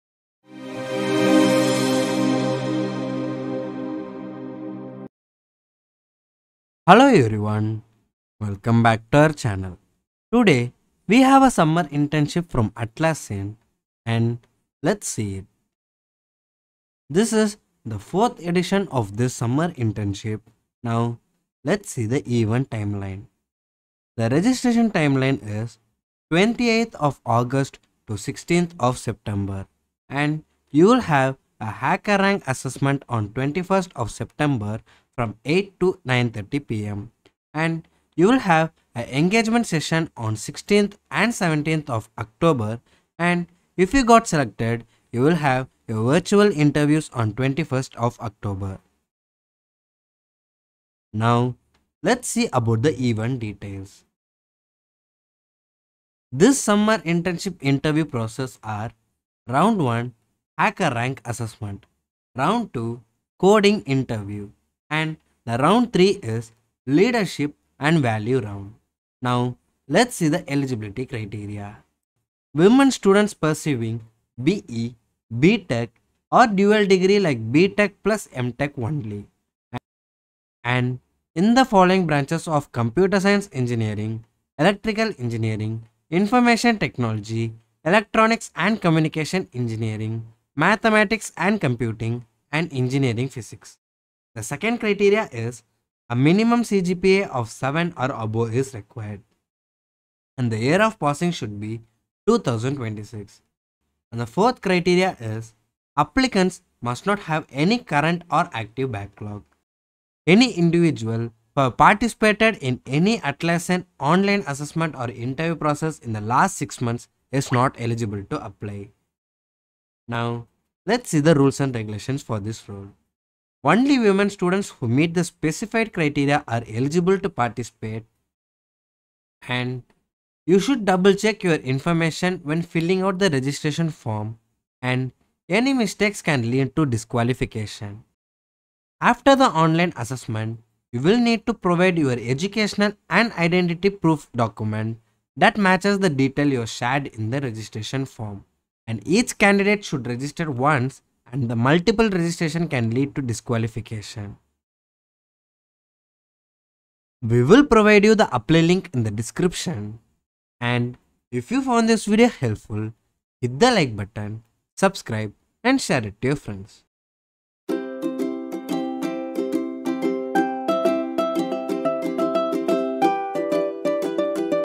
Atlassian's 4th Edition Summer Internship Program
The wait is over! We're thrilled to unveil the 4th Edition of Atlassian's "Women in Tech" event, exclusively designed to help you secure an exciting summer internship in 2025. This event has been a game-changer for women engineers, offering pathways to transformative careers in tech. 🌟

Event Highlights:

HackerRank Assessment: Test your coding skills in Round 1.
Coding Interview Round: Show your technical expertise in Round 2.
Leadership & Values Round: Demonstrate your fit for Atlassian's culture in Round 3.
Eligibility:

Degrees: B.E, B.Tech, Dual degrees in Computer Science, IT, ECE, and more.
CGPA: Minimum of 7 required.
Passing Year: 2026.
No Backlogs: Must have a clean academic record.
Join us to celebrate diversity, innovation, and collaborative success. Secure your spot for this life-changing opportunity and potentially land a pre-placement offer based on your performance!

🚀 Don't miss out! Register now and prepare to launch your career with Atlassian!